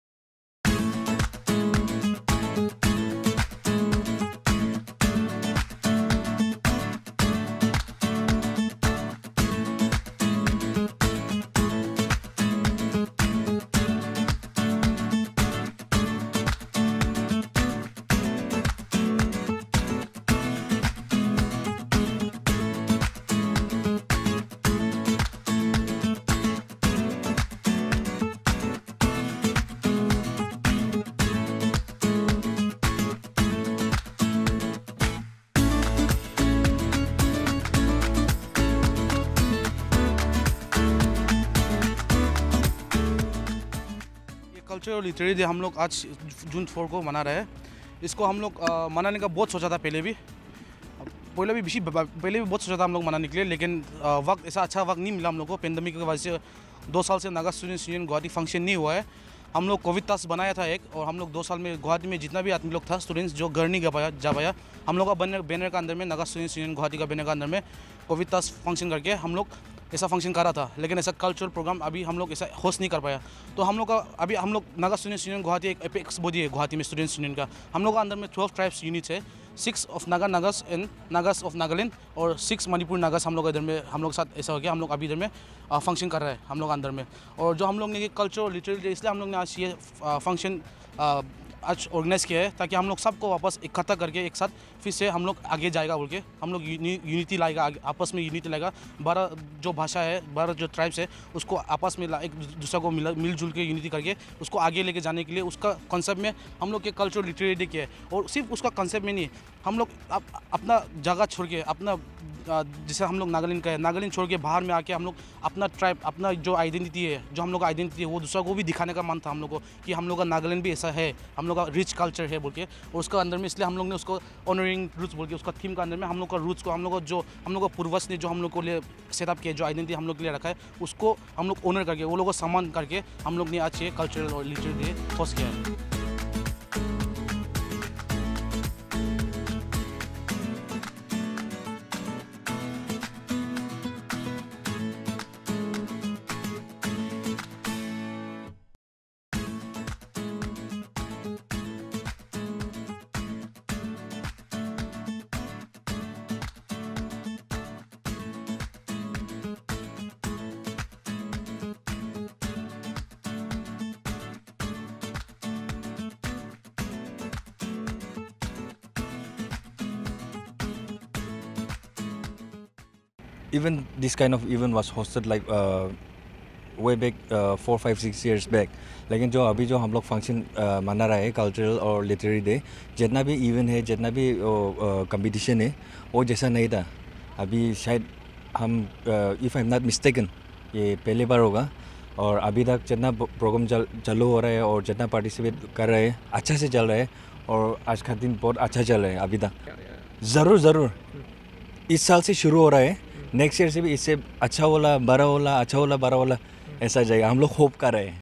GUWAHATI NAGA STUDENTS UNION CELEBRATES CULTURAL CUM LITERARY DAY AT THE SHILPAGRAM, PANJABARI
The Guwahati Naga Students Union on Saturday observed Cultural cum Literary Day at the Shilpagram, Panjabari. With the theme of the program being “honouring roots” the Ethnicity of Nagas and cultural heritage were at full display on this day. With regards to the program, the Guwahati NSF stated that this sort of program was the first of its kind in Guwahati and claimed that the rich cultural heritage of Nagas were on full display. It is likely that more of these events will take place in the coming days